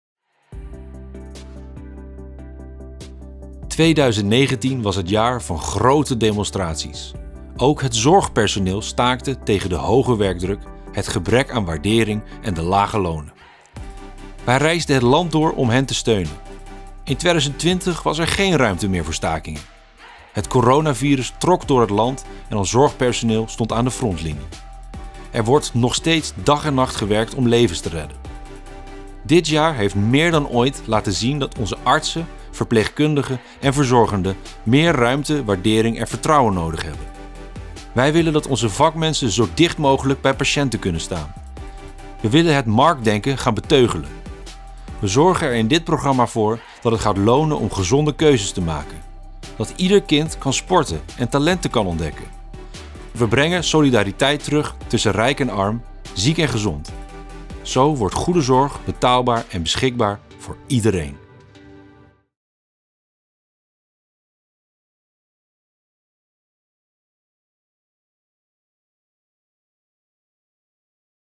Goede zorg, daar is toch iedereen bij gebaat? | ONS PLAN VOOR EEN EERLIJKE TOEKOMST
Je weet nooit wanneer je liefdevolle zorg nodig hebt. Maar je moet er op kunnen rekenen als het zover is. De zorg moet betaalbaar zijn en beschikbaar voor iedereen. En we moeten gezond leven stimuleren.